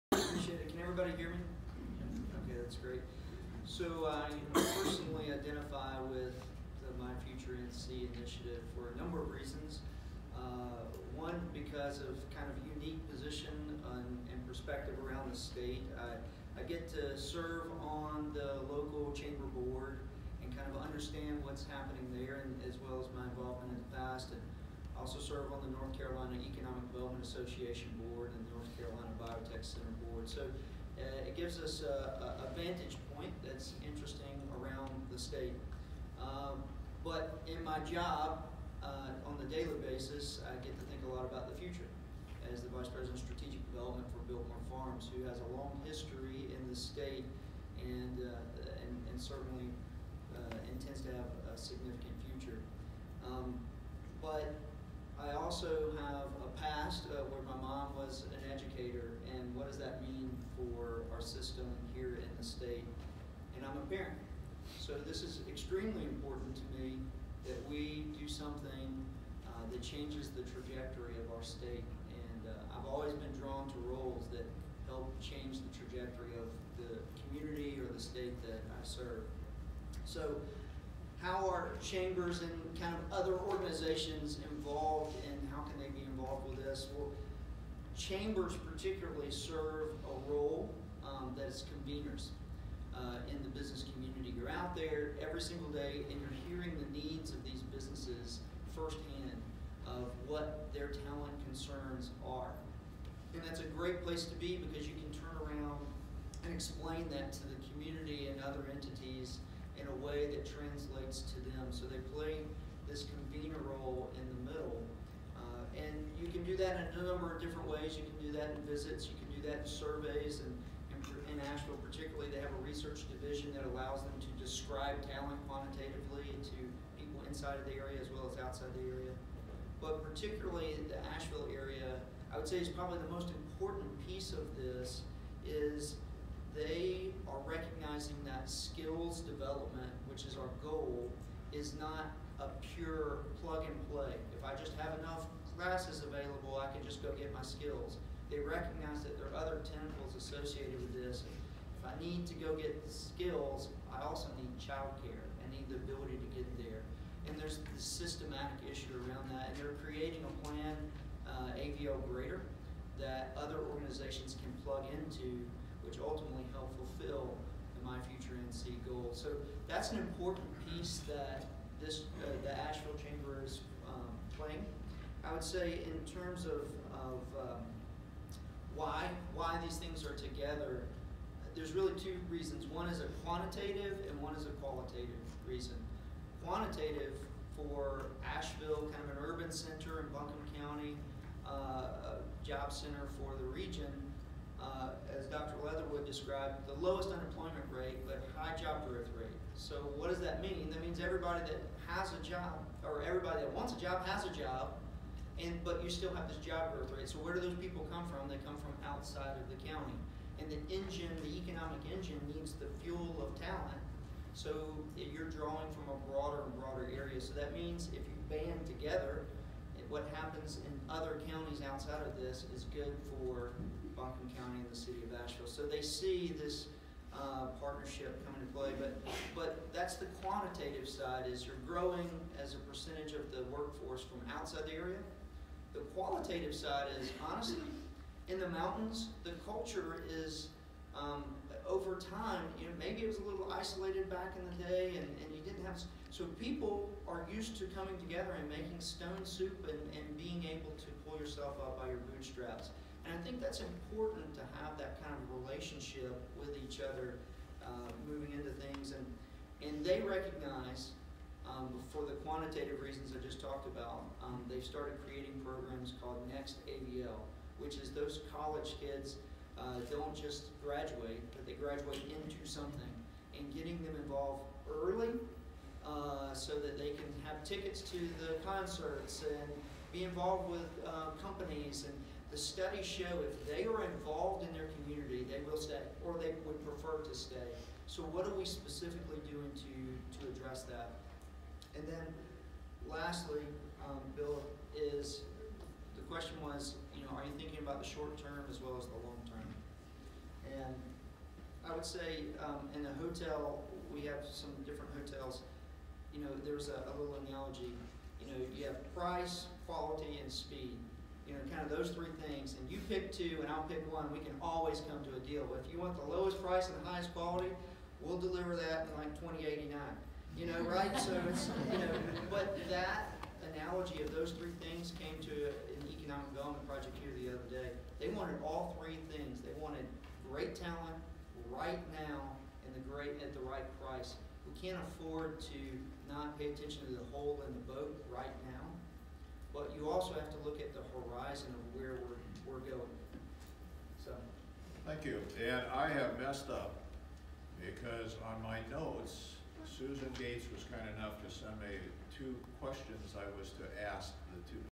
NC Chamber Ed & Workforce Conference - Part 3
Ben Teague, Vice President for Strategic Development with Biltmore Farms shares why My Future NC's postsecondary credential attainment goal is important for employers in Western North Carolina.